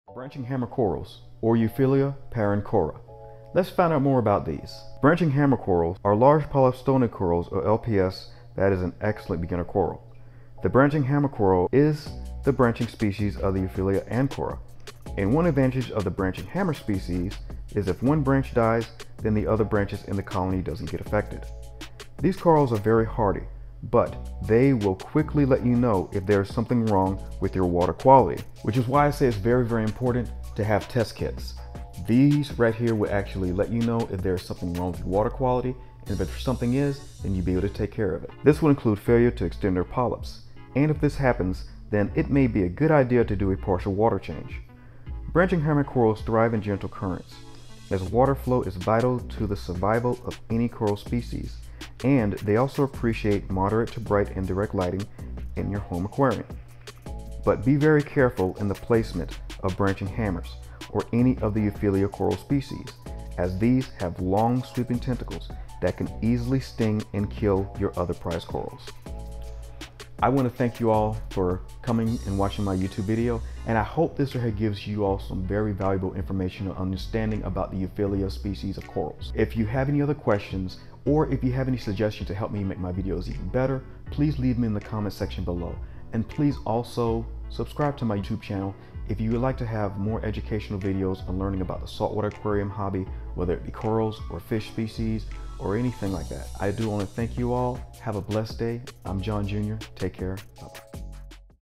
Beginners guide Branching Hammer corals
Hammer Time; Hammer corals from the family of Euphylia are beautiful L.P.S or large polyp stony corals that are excellent for beginners, and are very hardy. reef-tank saltwater aquarium hammer lps corals beginner corals branching hammer corals